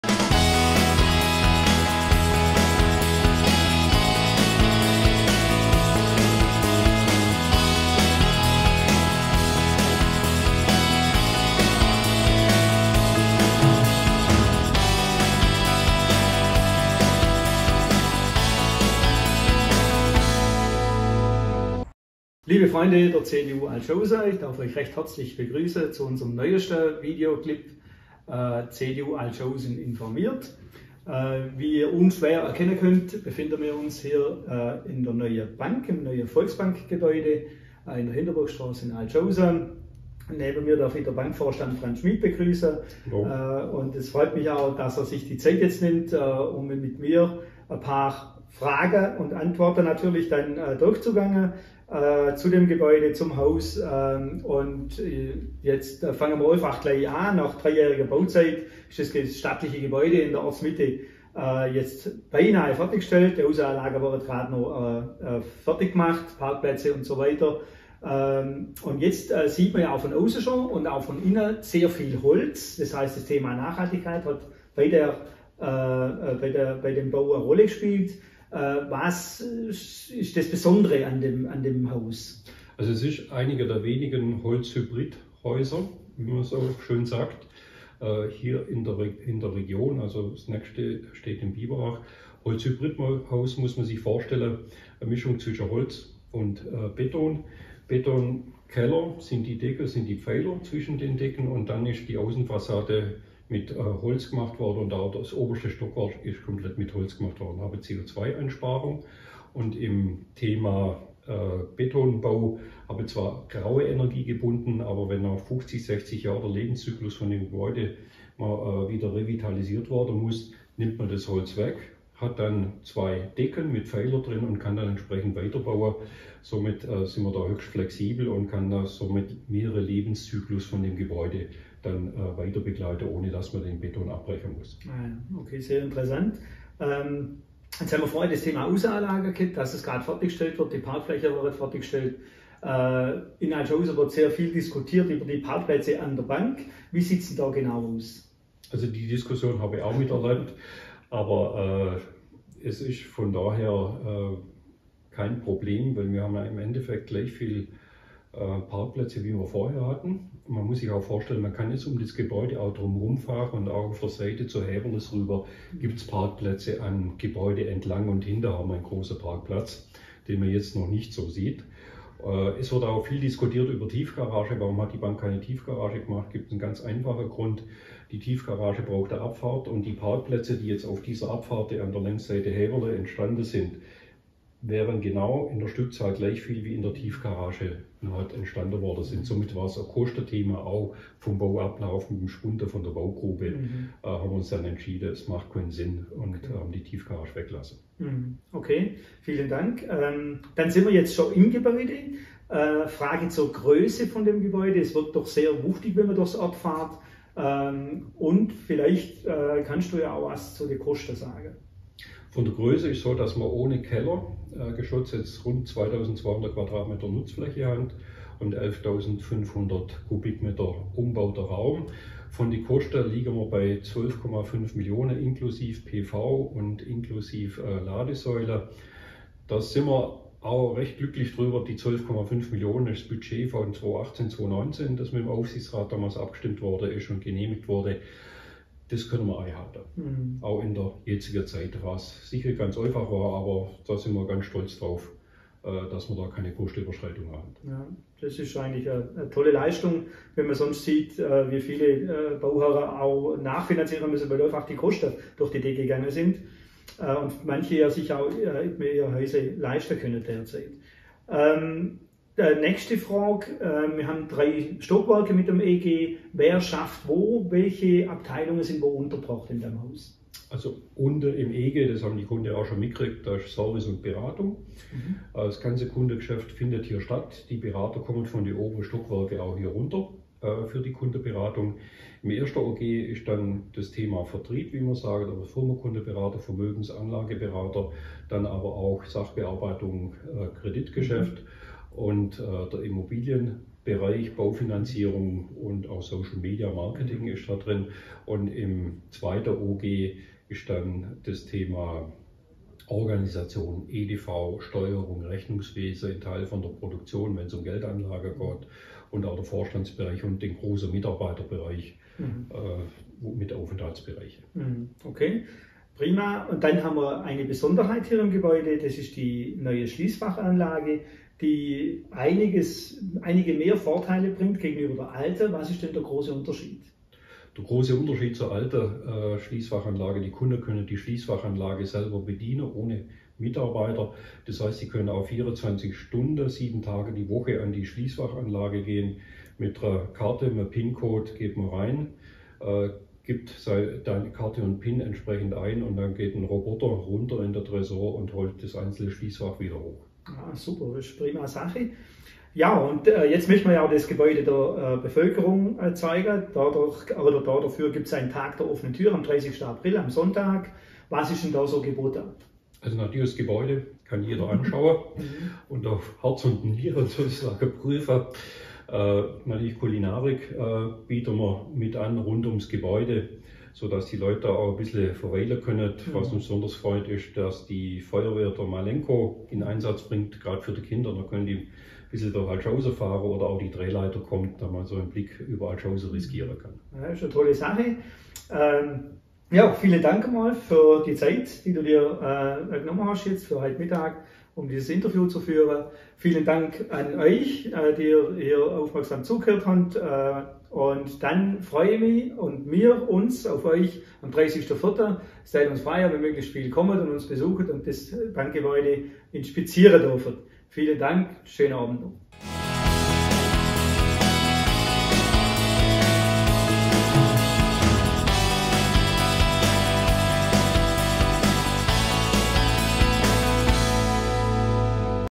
CDU Altshausen informiert... zum neuen Volksbankgebäude: HAUS DER IMPULSE
In diesem Video interviewt der CDU-Gemeinderat Martin Kiem den Vorstand der Volksbank Altshausen, Franz Schmid, zum neuen Volksbankgebäude.